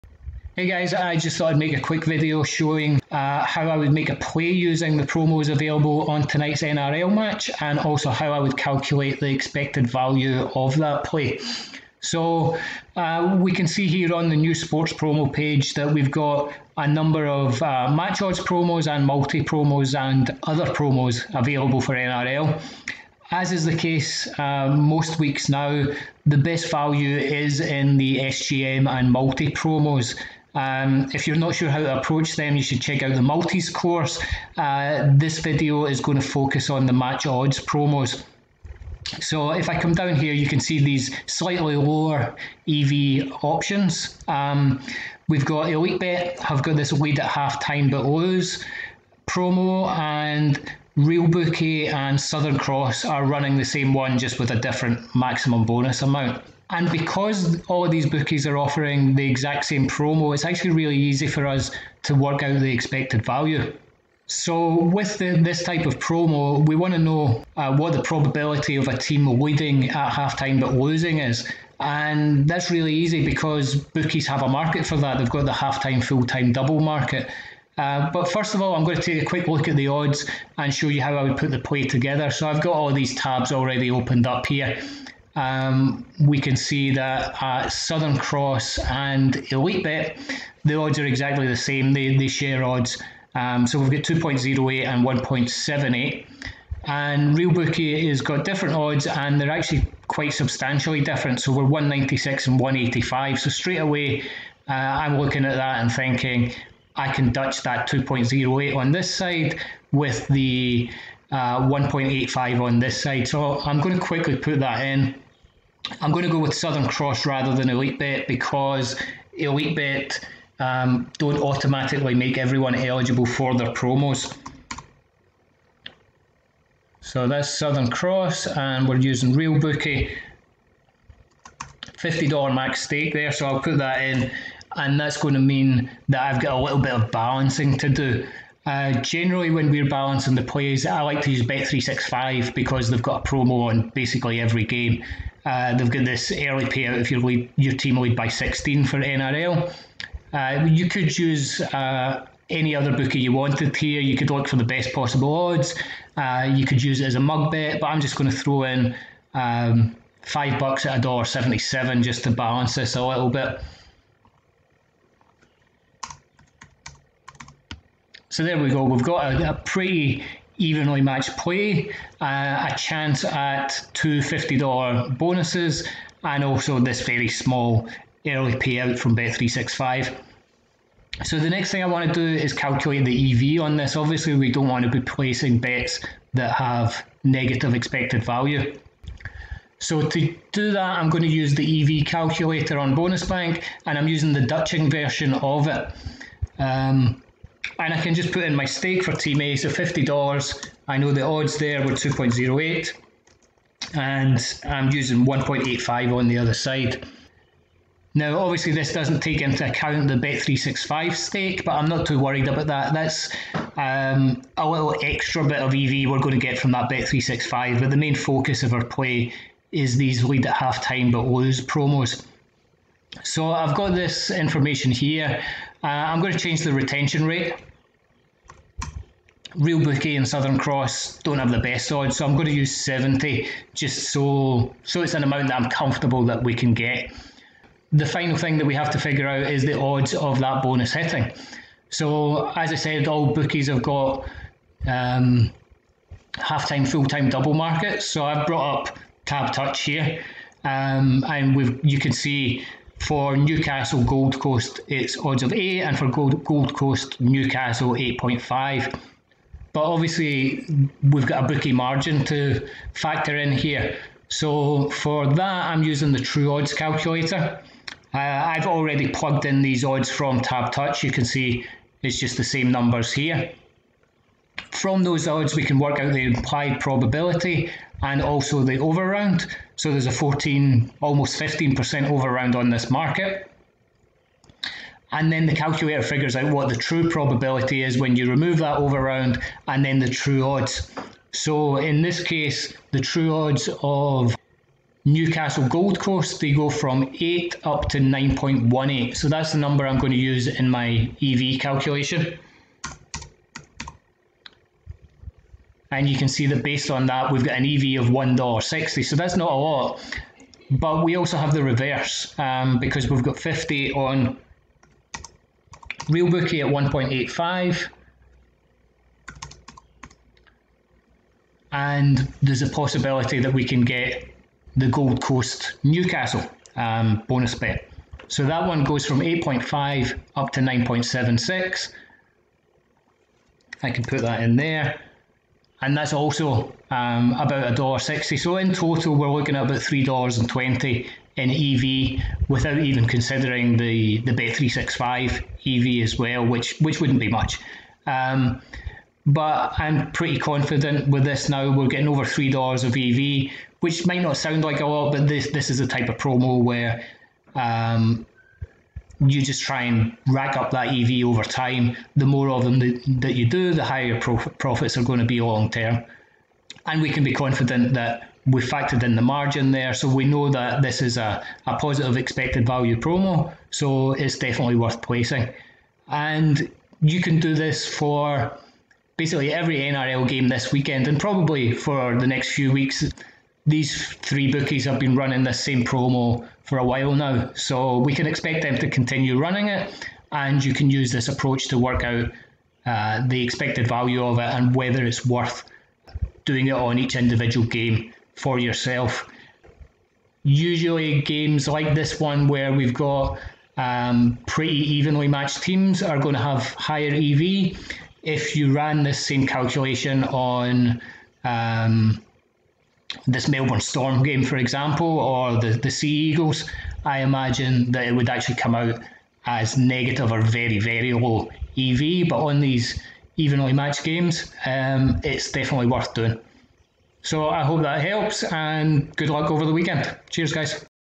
Calculating EV of a "Lead at HT but lose" promo
Success in MatchedBetting is all about placing bets with a positive expected value. This is done by taking advantage of bookie promos. This video shows how you can create a +EV play for NRL, and also how to calculate the EV.